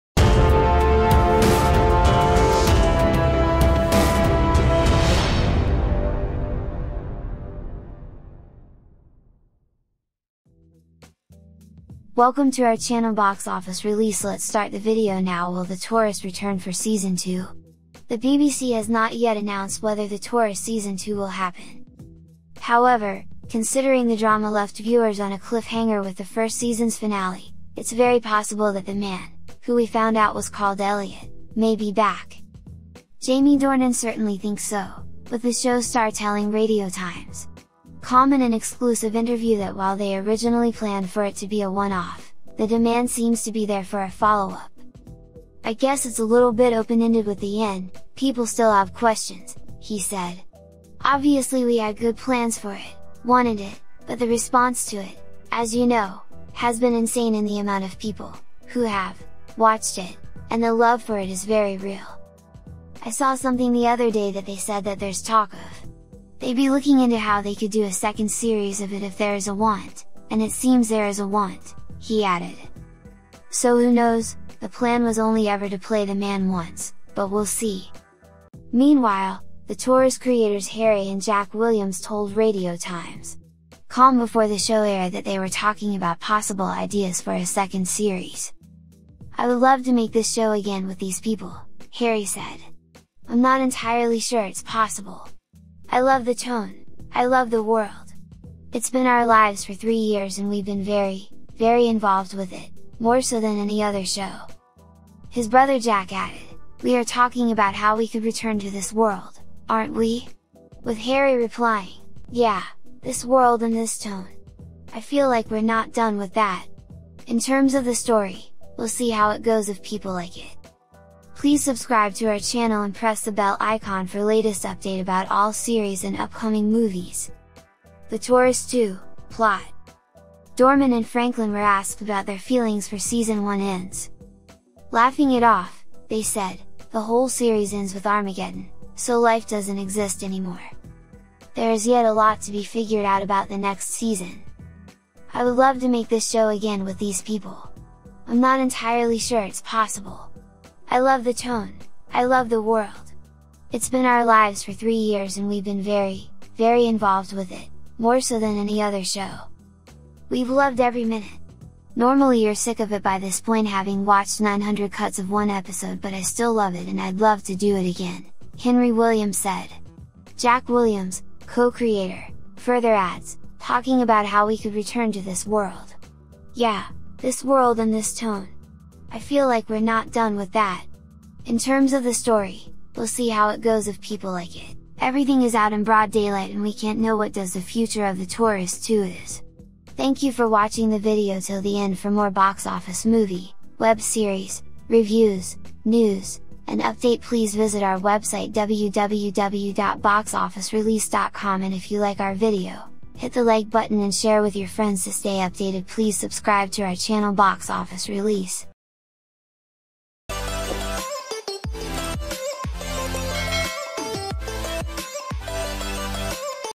The Tourist Season 2 Will it return? and Plot- Box Office Release
The Tourist Season 2 fans and followers wants to Know everything about The Tourist Season 2 Release Date, Cast, Plot, Trailer & more.

However, considering the drama left viewers on a cliffhanger with the first season's finale, it's very possible that The Man – who we found out was called Elliot – may be back.
Jamie Dornan certainly thinks so, with the show's star telling RadioTimes.com in an exclusive interview that while they originally planned for it to be a one-off, the demand seems to be there for a follow-up.